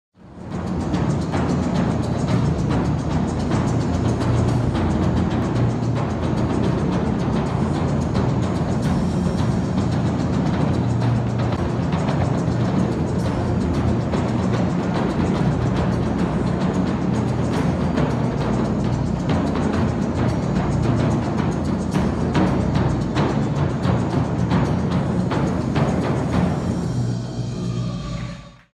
A giant drum with a robotic twist
It could be seen as an aerial drum but it is not. The drummer is just hanged on top of a giant robot arm.
Quite spectacular and ideal for any big event.
Ref: musique_334